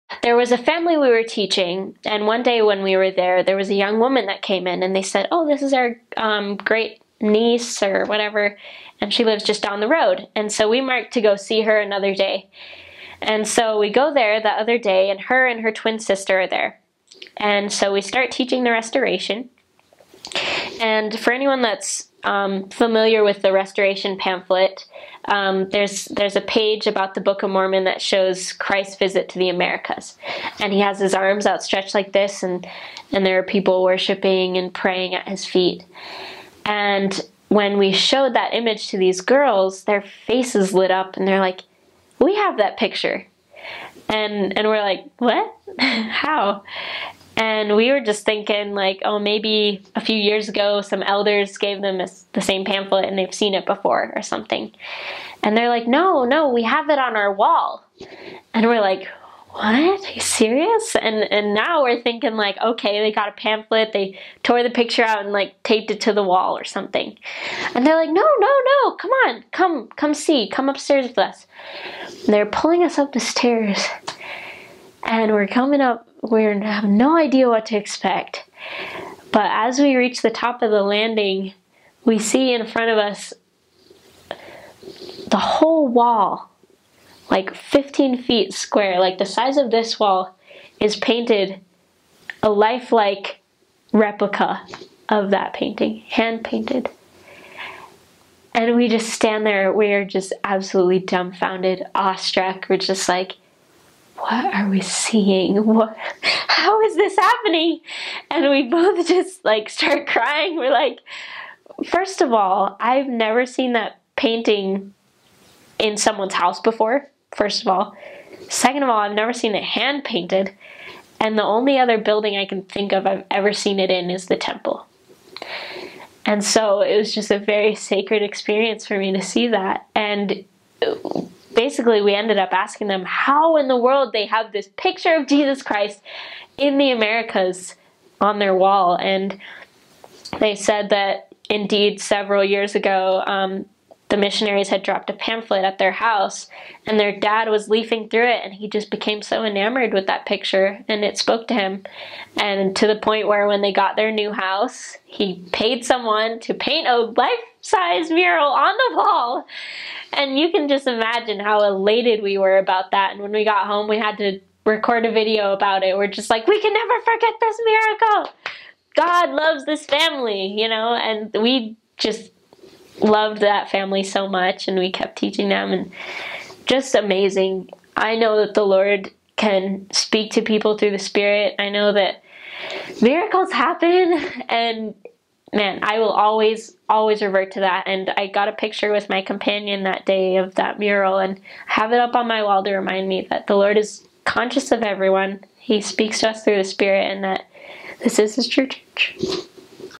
Finding a Huge Mural of Christ Visiting the Americas, in a Non Member Home
Finding a Mural of the Picture of Christ Visiting the Americas, in a Non Member Home (LDS).